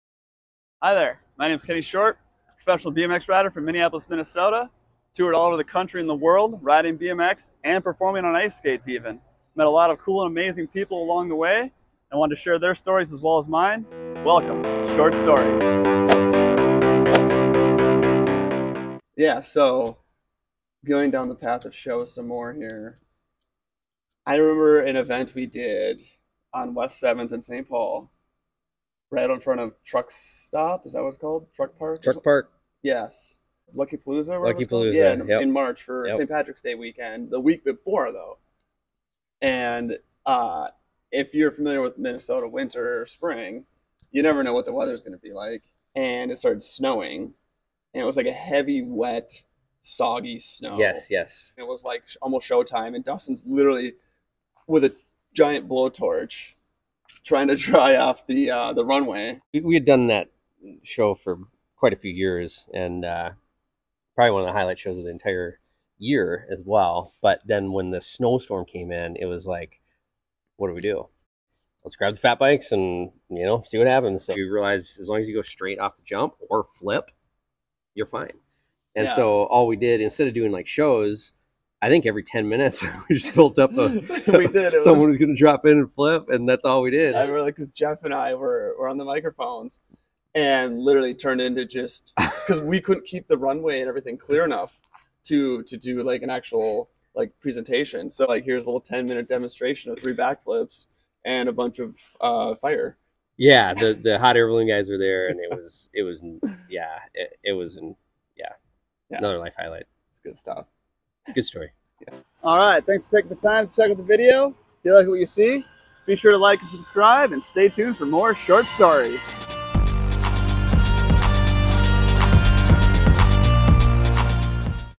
Ride MN, West 7th Snow Show
In this episode I catch up with Dustin Grice, owner and creator of Ride MN and we talk about the West 7th event where we executed BMX shows in a snow storm!